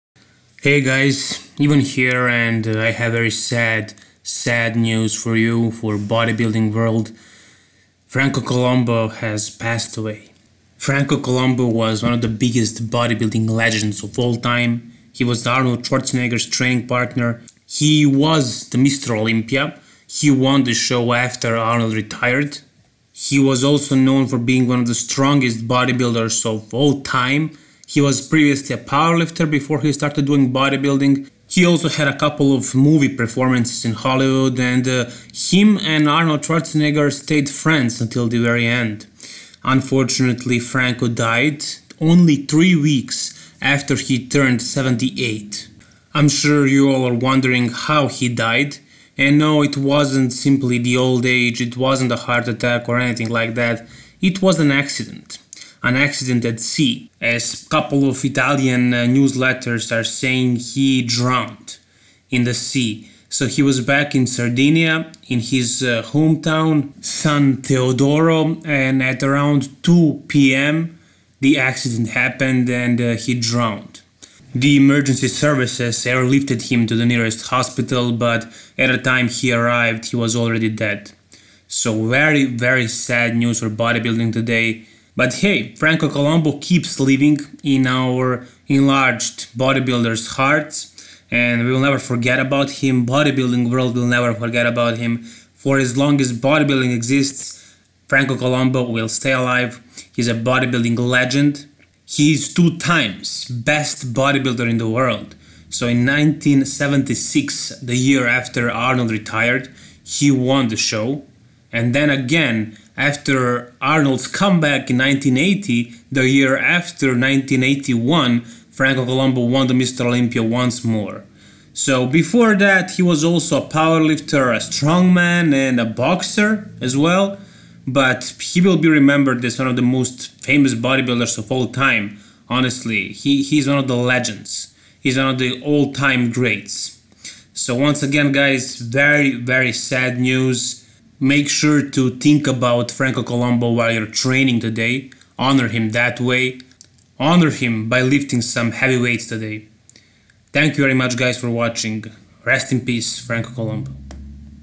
Bodybuilding Legend Remembered | 2022
Body building legend Franco Columbu has passed away. He will be missed.
The former bodybuilder/actor died suffering a heart attack while swimming.
#1 on trending#FrancoColumbu#bodybuilding#Franco Columbu R.I.P - MESSAGE FROM ARNOLD SCHWARZENEGGER#Franco Columbu R.I.P 🙏 - Tribute Video